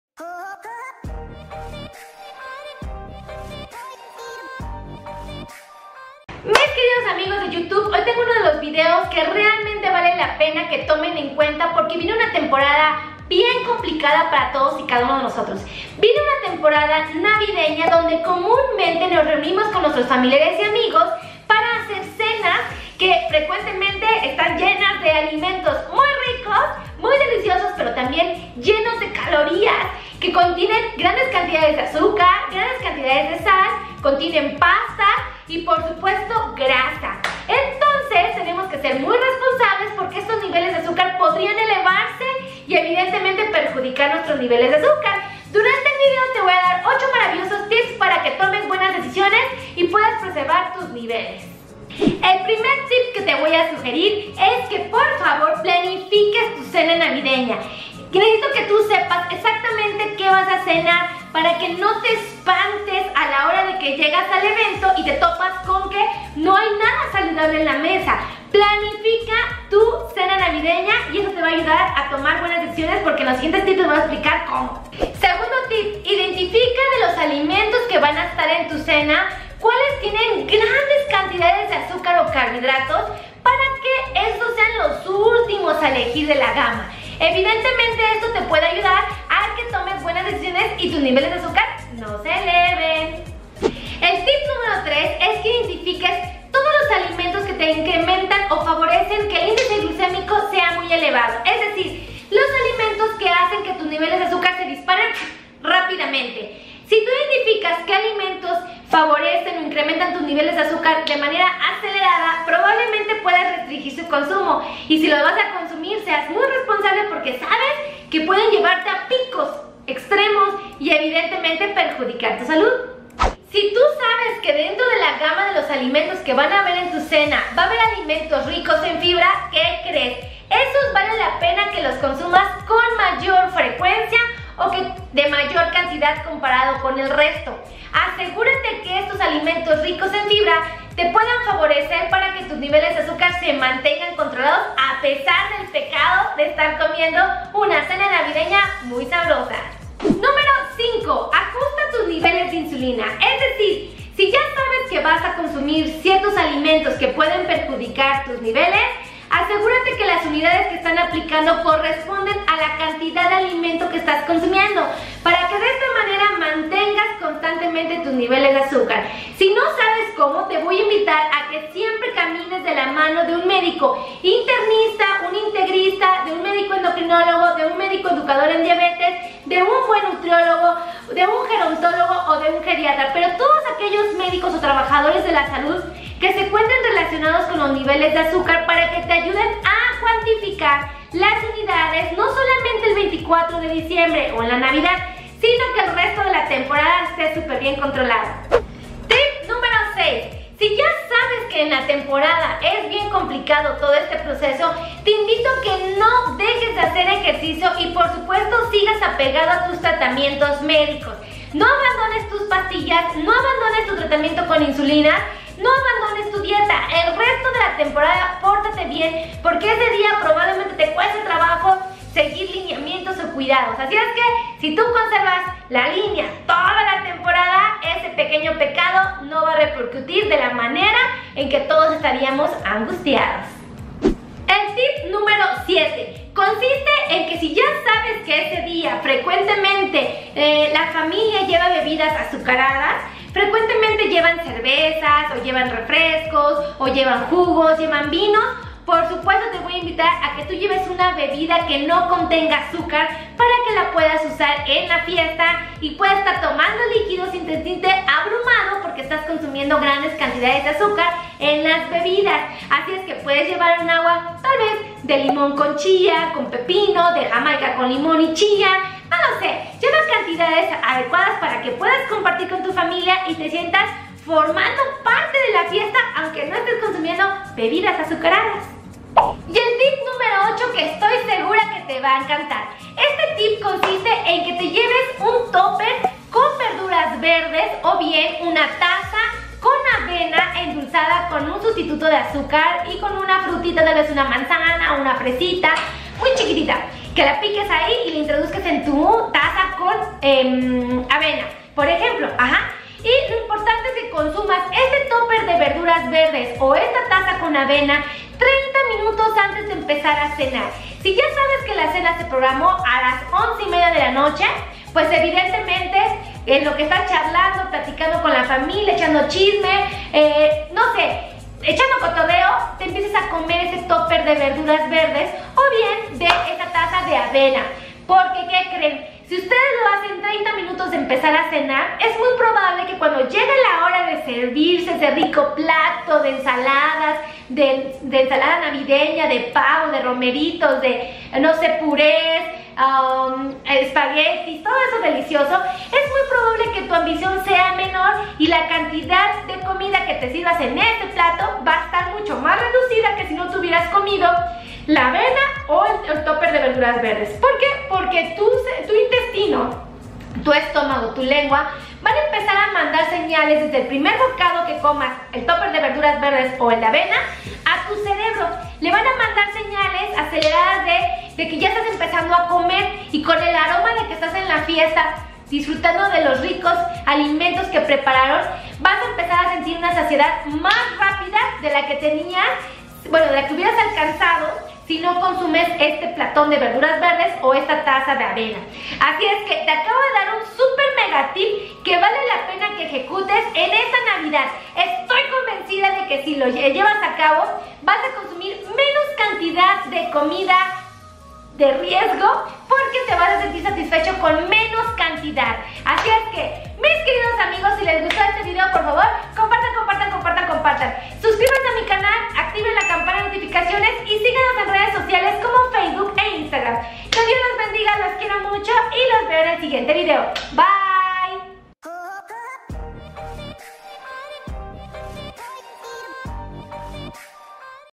8 TIPS para CUIDAR TU AZUCAR esta navidad. 🎂🍾⁄▶Dra. Tejeida Melissa
Consultas: 📞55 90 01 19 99📞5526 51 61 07  en el 🏢World Trade Center Ciudad de México
Consejos para el cuidado de pacientes que viven con diabetes